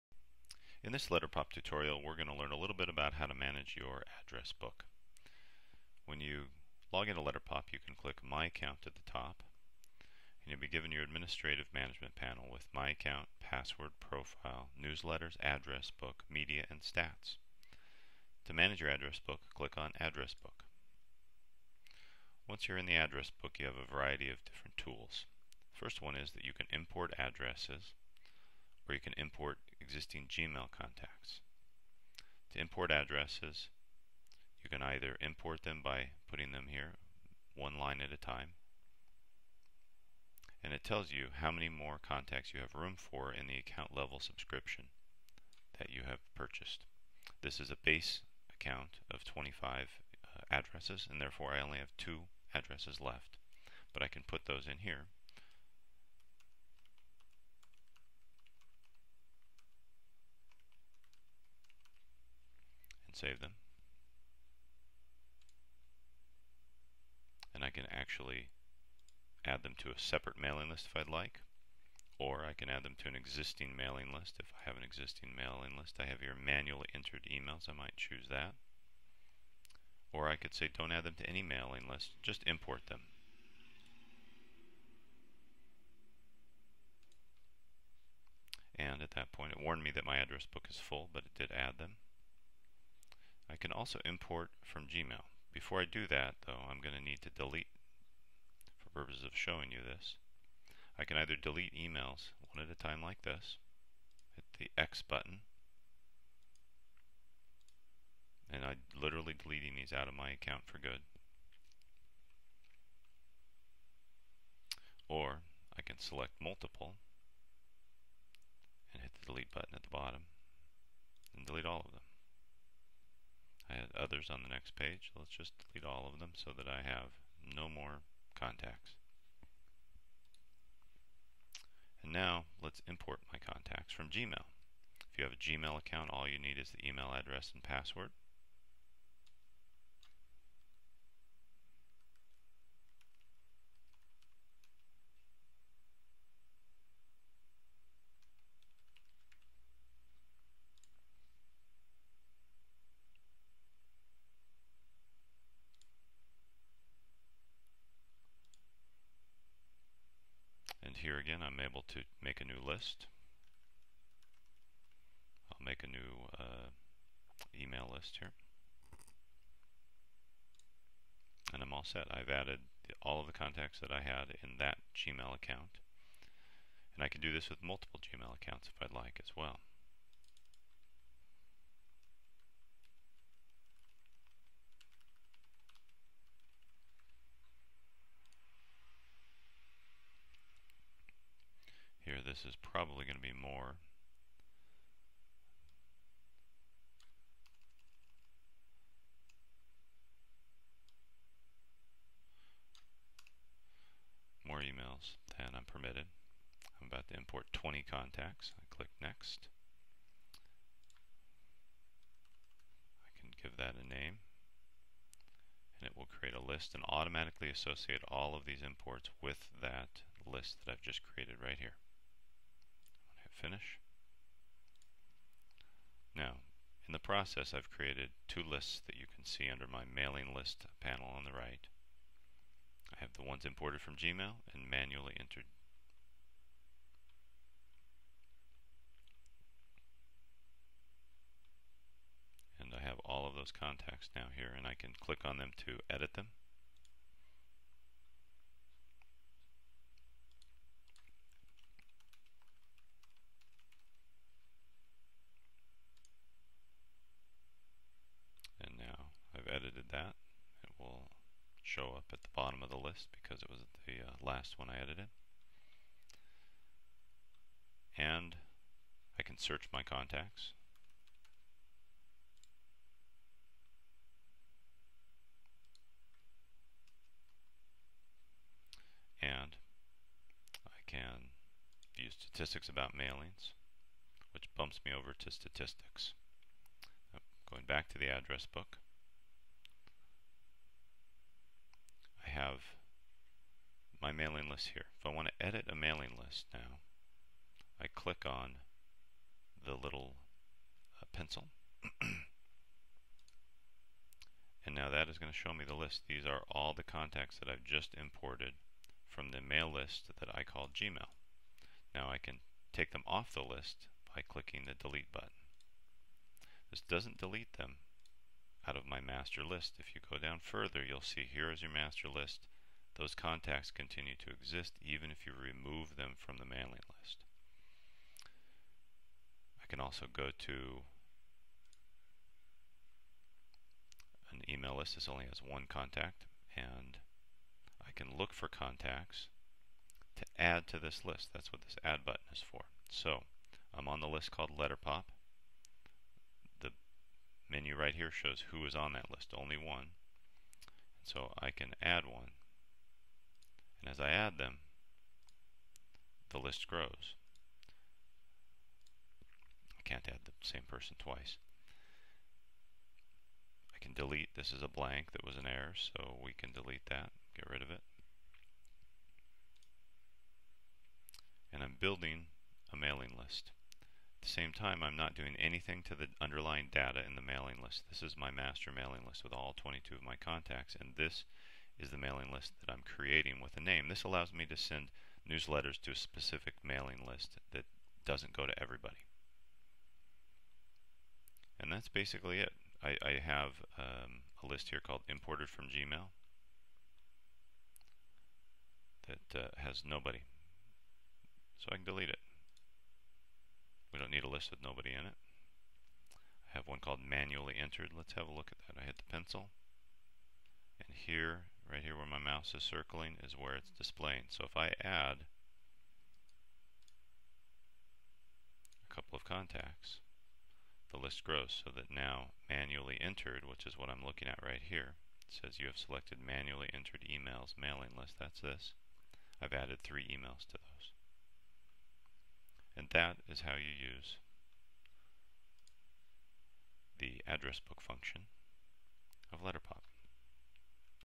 How to Manage Your Address Book in LetterPOP
A tutorial on how to manage your address book in LetterPOP.com, newsletter creator tool.